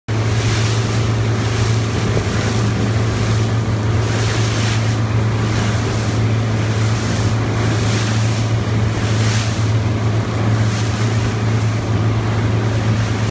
Wightlink's Victoria of Wight Off The Isle Of Wight | Isle of Wight Radio
Wightlink's new Victoria of Wight spotted off the south coast of the Isle of Wight.